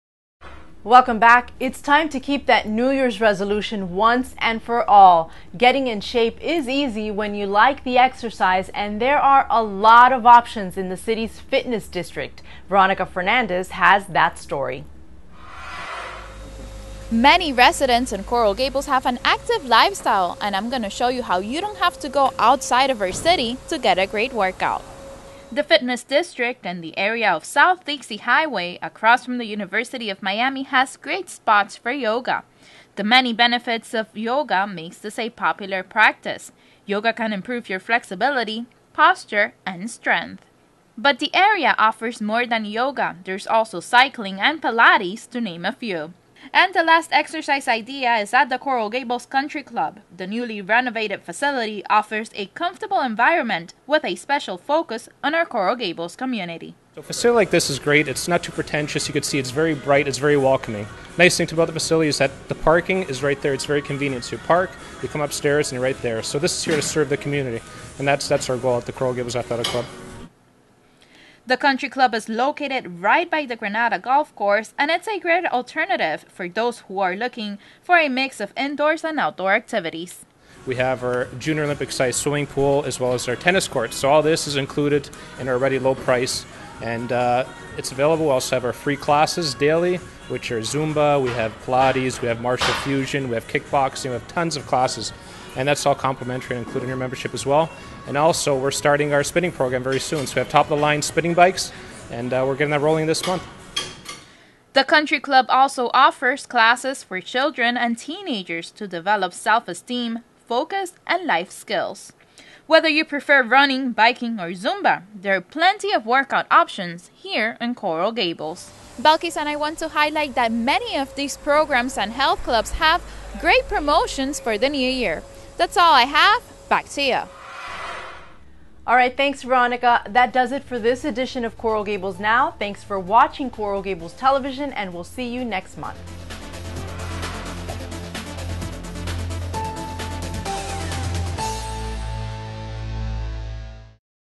Coral Gables Athletic Club on Coral Gables Television!
Check out the newly renovated facilities at the Coral Gables Athletic Club as seen on Coral Gables TV!